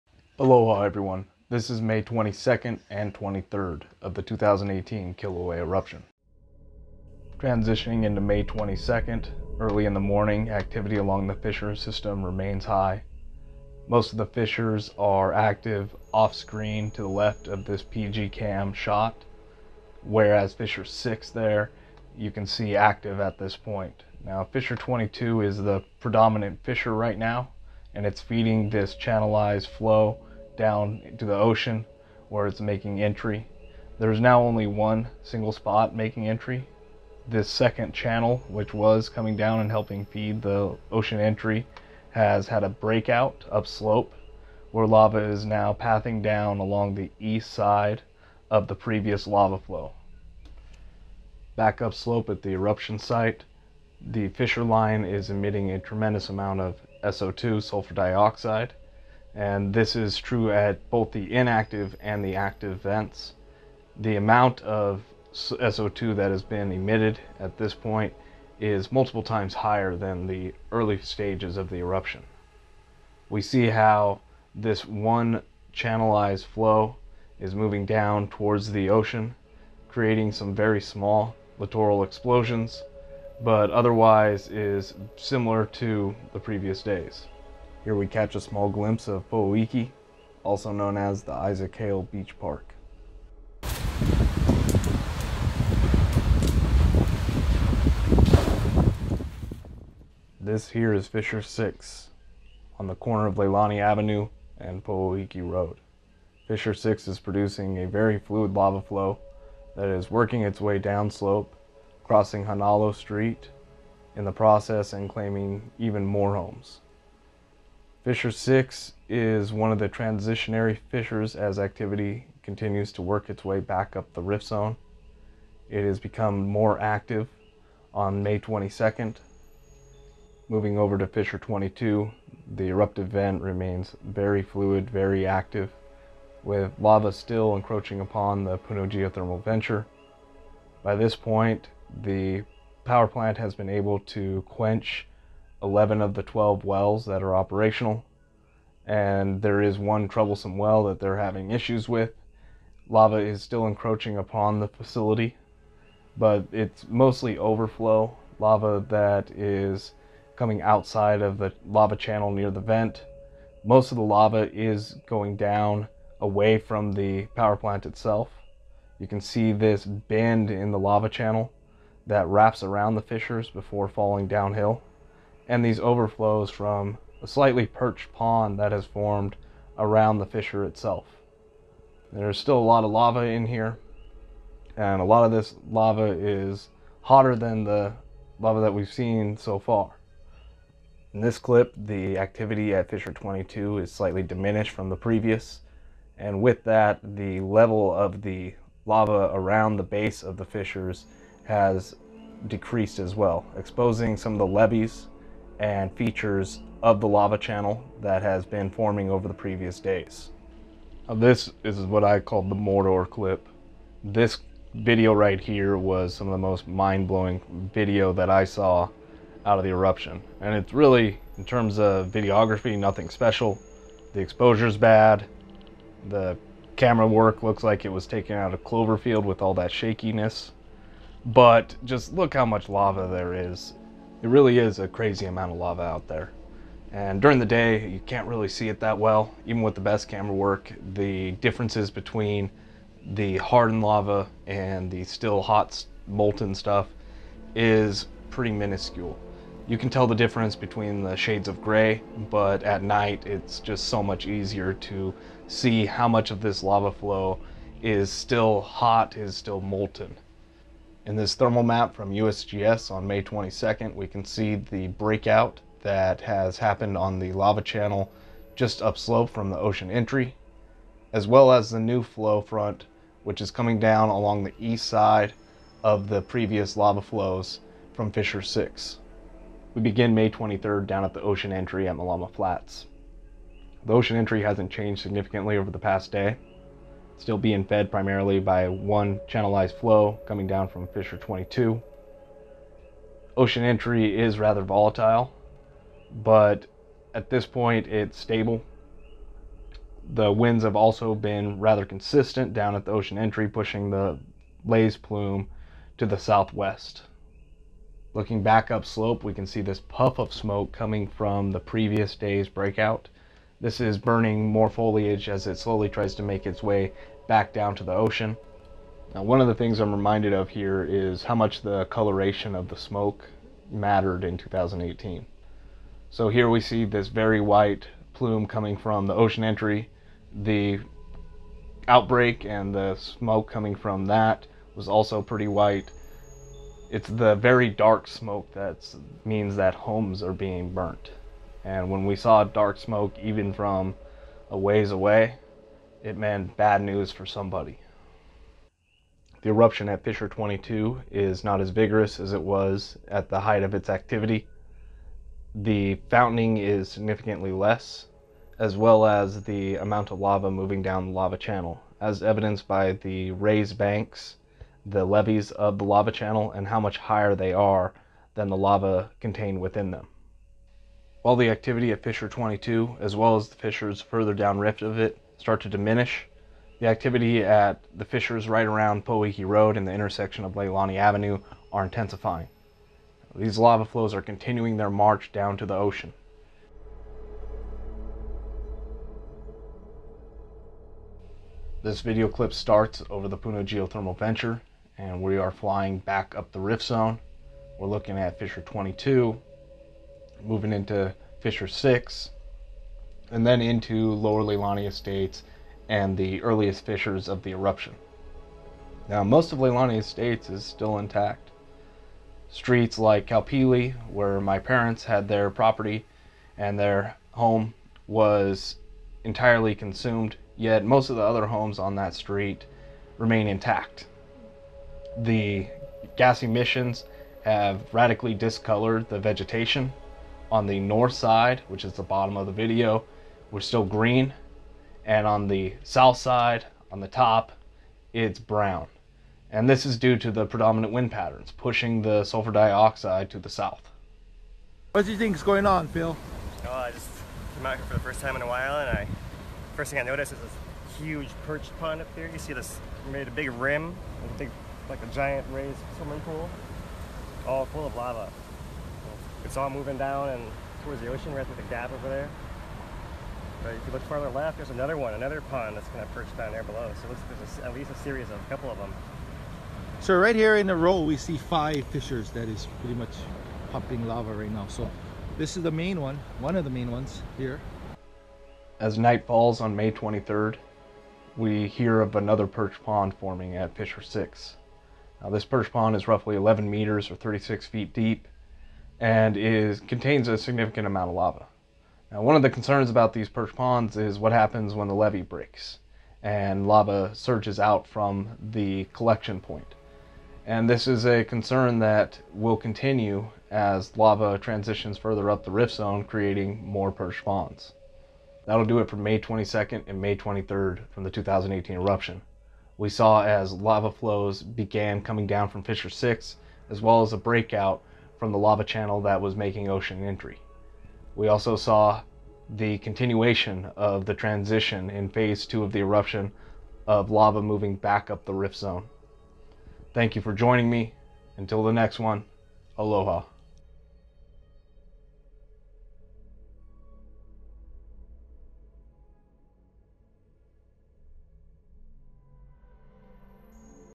Drones On - May 22nd and 23rd, 2018 Kilauea Eruption - Perched Ponds
Combined footage of the 2018 Kilauea Eruption, May 22nd and 23rd. In this episode, the eruption continues to feed an ocean entry by MacKenzie State Park as activity slowly works it way up the rift zone. Perched ponds of lava have collected around a few of the active fissures.

This series tells one version of the 2018 Kilauea eruption on the Lower East Rift Zone, where lava erupted in a residential neighborhood. The story is told through available footage from many sources, including USGS, Hawaii Count Civil Defense, DoD, National Guard, DLNR, and private citizens. This series will go through each day of the eruption and tell the story from the lens of a Leilani Estates resident.

Footage in this episode courtesy of USGS, DOD, Hawaii County Civil Defense, Mick Kalber, Scott Wilson, Ikaika Marzo, and Andrew Hara.